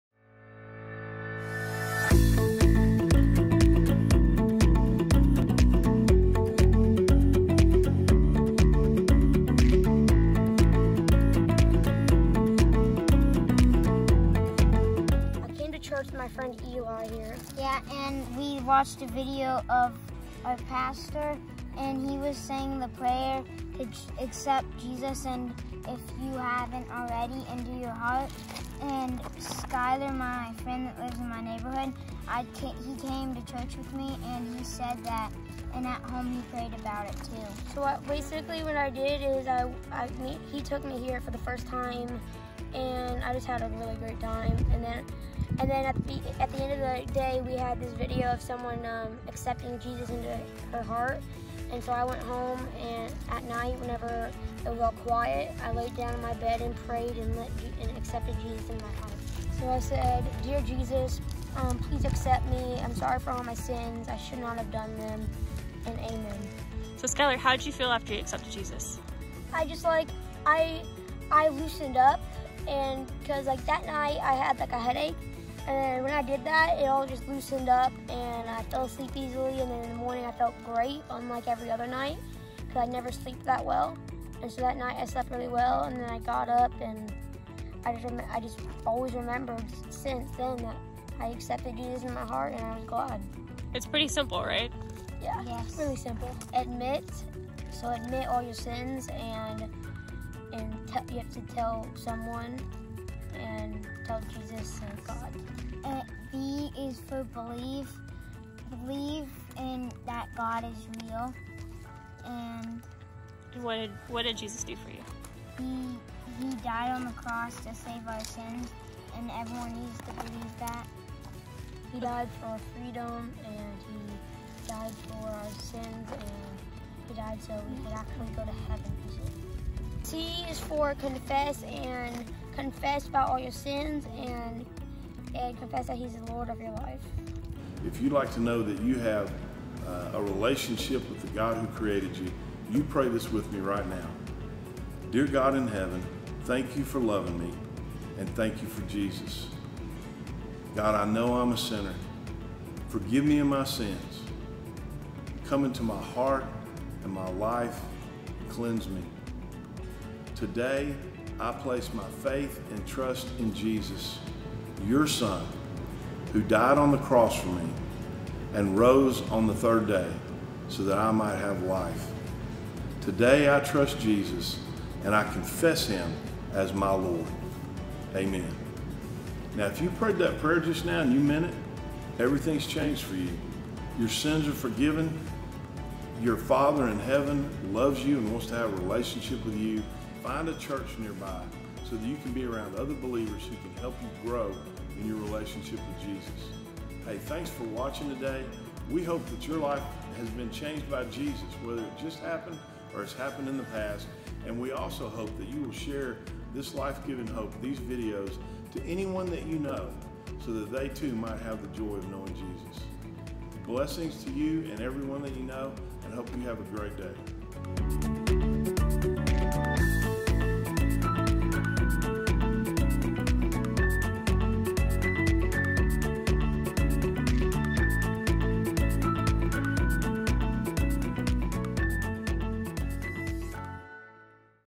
Skyler's Story
As Skyler heard the story of a woman named Brittany, and how she found peace in Jesus, Skyler realized he wanted a relationship with Jesus, too. Check out his story!